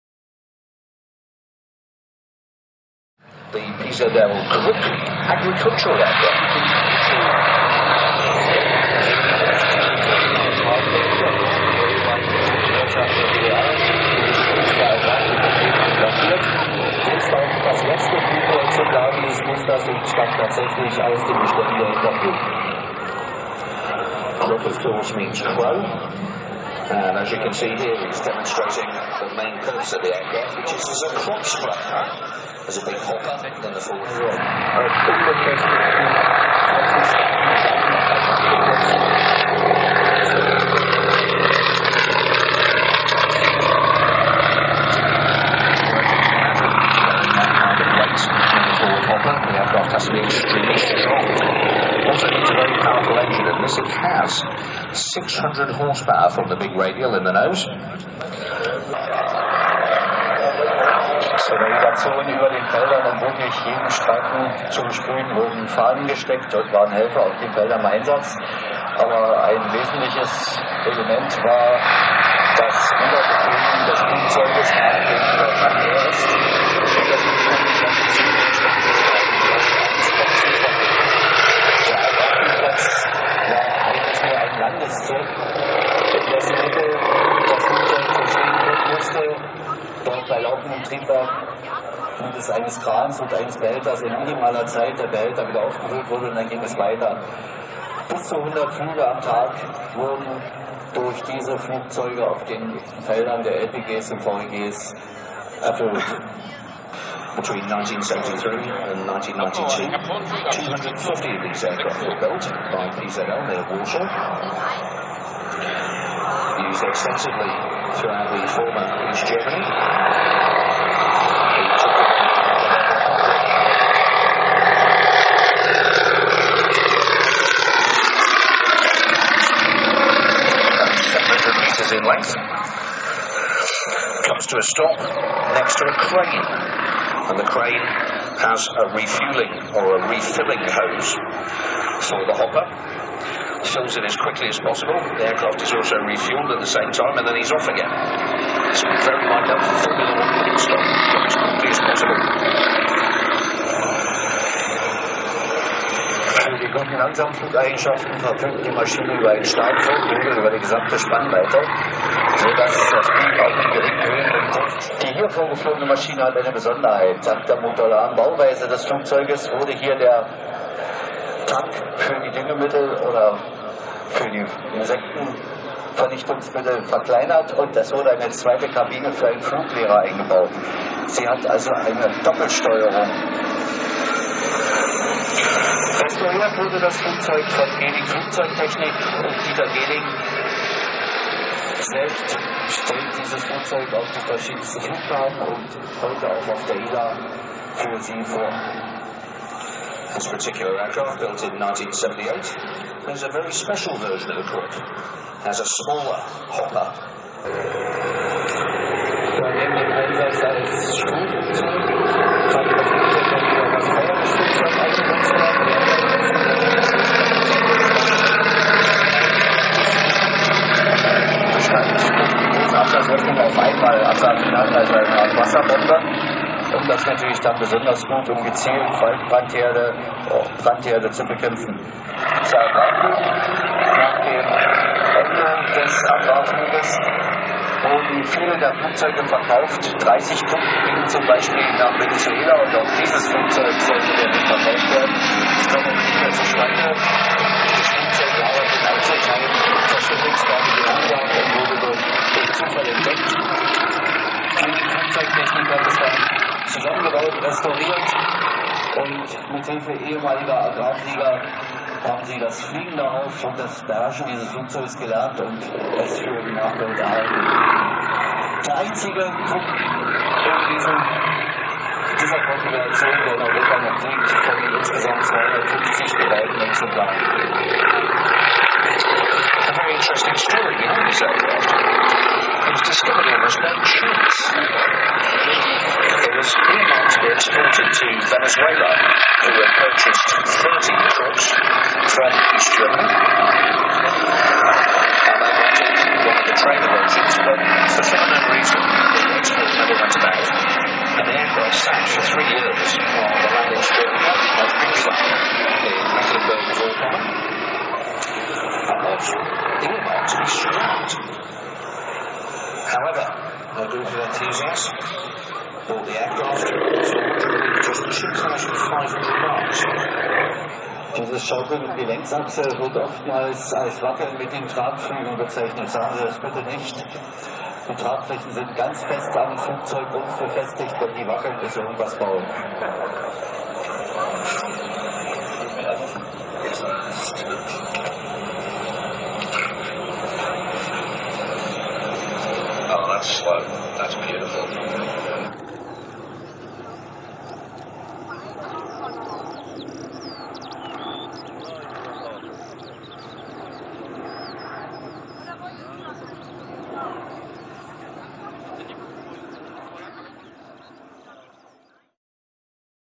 PZL 106 KRUK (CROW), THE LAST FLYING KRUK IN ACTION!
Full Demo of the last flying PZL 106 Kruk Doubleseater at the ILA Berlin Airshow 2014, flown by the Owner Dieter Gehling. The Kruk (Crow) was a Waterbomber and Agricultural Aircraft and in Service of the ex-DDR/GDR Airline "Interflug".